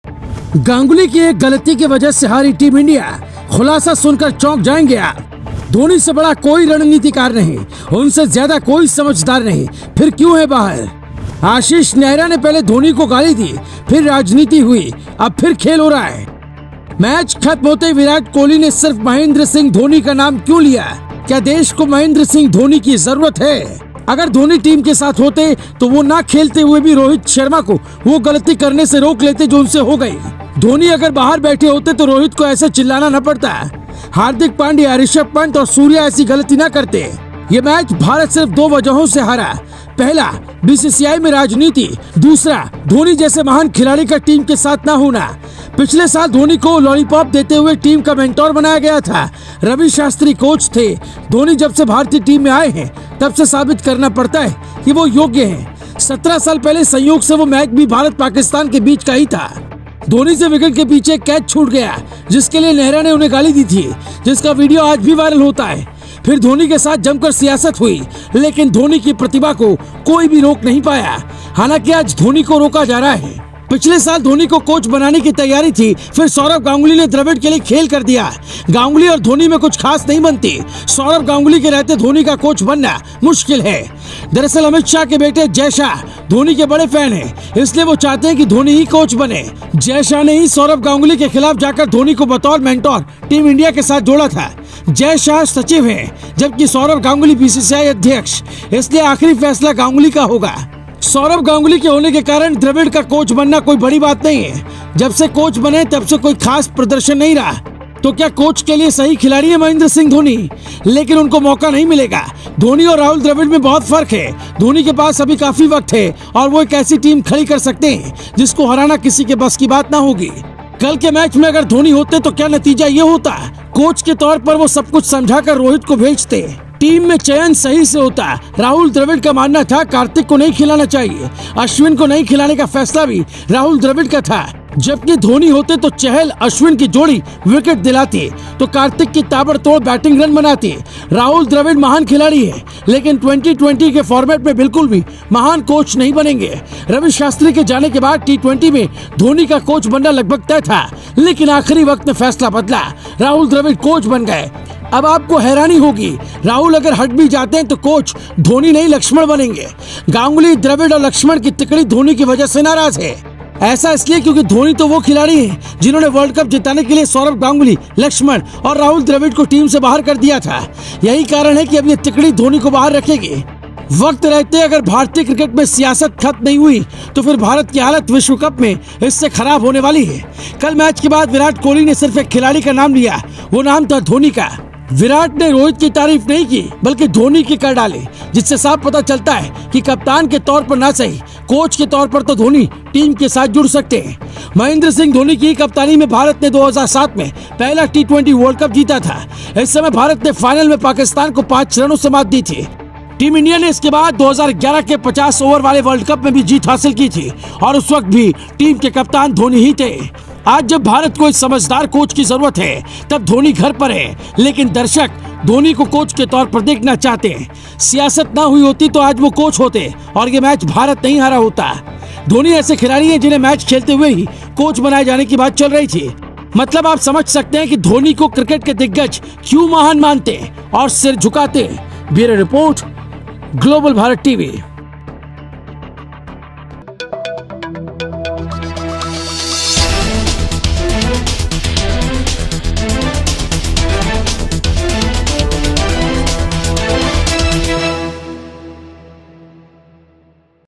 धोनी को बनाया जाएगा कोच? धोनी जैसा महान क्यों बैठा है बाहर? हार के बाद कोहली ने लिया धोनी का नाम!
मैच ख़त्म होते ही विराट कोहली ने सिर्फ महेंद्र सिंह धोनी का नाम क्यों लिया? क्या देश को महेंद्र सिंह धोनी की ज़रूरत है? अगर धोनी टीम के साथ होते तो वो न खेलते हुए भी रोहित शर्मा को वो गलती करने से रोक लेते जो उनसे हो गई! धोनी अगर बाहर बैठे होते तो रोहित को ऐसे चिल्लाना न पड़ता, हार्दिक पांड्या, ऋषभ पंत और सूर्या ऐसी गलती न करते? ये मैच भारत सिर्फ दो वजहों से हारा? पहला BCCI में राजनीति? दूसरा धोनी जैसा महान का खिलाड़ी टीम के साथ न होना, पिछले साल धोनी को लॉलीपॉप देते हुए टीम का मेंटॉर बनाया गया था